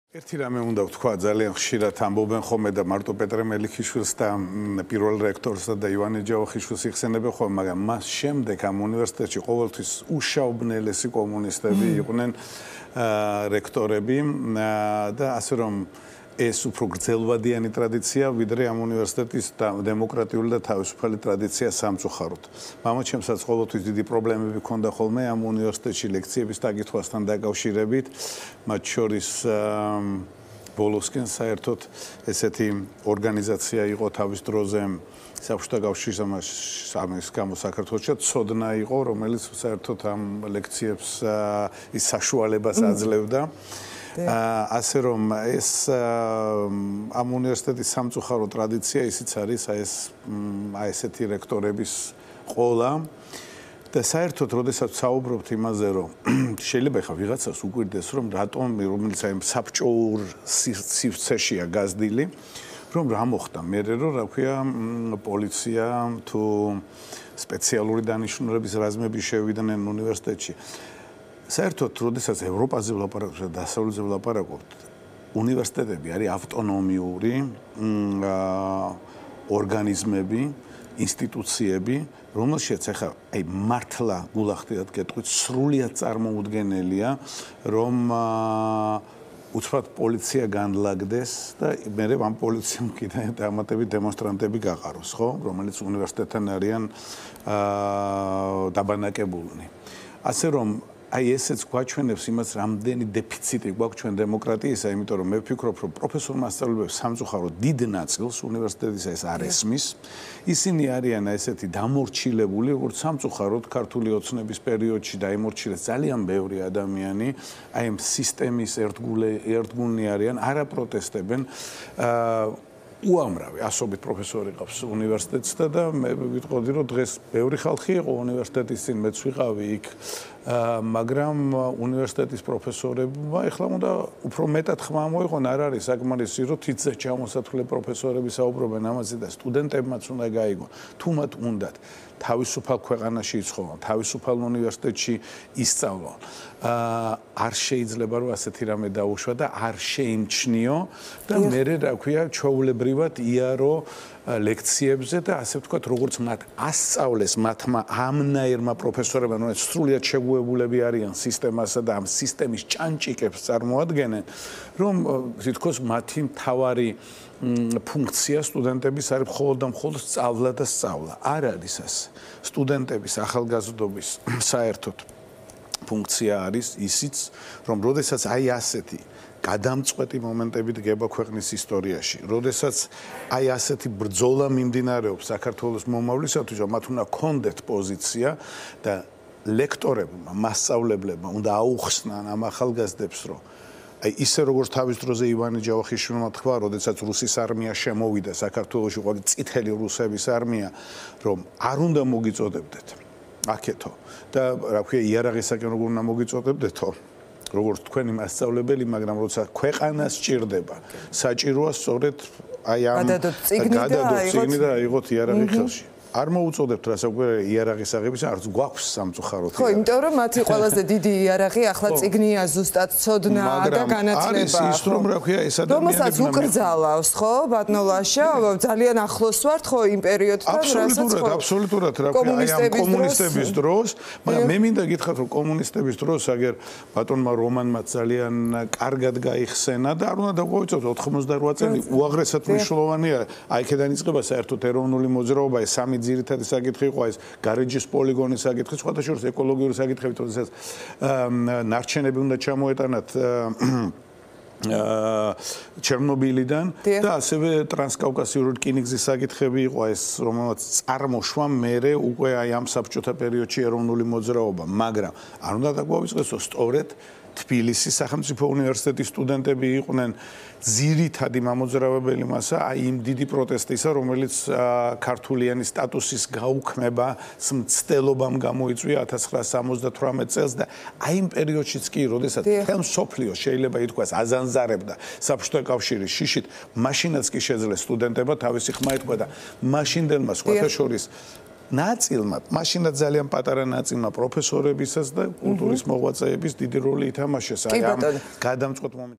ლაშა ბაქარაძე | კომენტარი
▪️ „სტუდენტებმაც უნდა გაიგონ, თუ მათ უნდათ თავისუფალ ქვეყანაში იცხოვრონ, თავისუფალ უნივერსიტეტში ისწავლონ არ შეიძლება ასეთი რამე დაუშვან“ — ლაშა ბაქრაძე